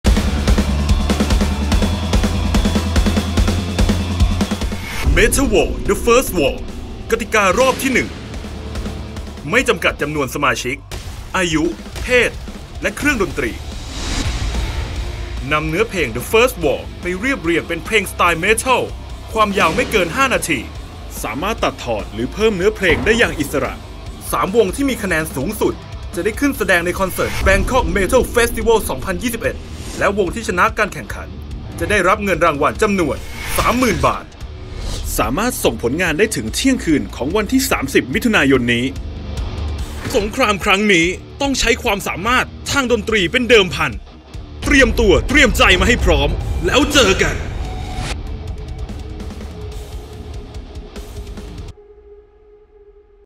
METAL WAR กับ กติกาการแข่งขันรอบแรก
METAL WAR 2021
สมัครแข่งขัน METAL WAR 2021 พร้อมอ่านกติกาเพิ่มเติมได้แล้ววันนี้
1.METAL WAR : THE FIRST WAR
รายการแข่งขันวง “เมทัล” ครั้งแรกในประเทศไทย
2.IDOLCORE : ไอดอลคอร์
รายการค้นหาไอดอลสาวที่รักการร้องและเต้น เป็นจังหวะ “เมทัล”
ทั้งสองรายการเปิดออดิชั่น วันนี้ - 30 มิ.ย. นี้
Line@ : @BananaMax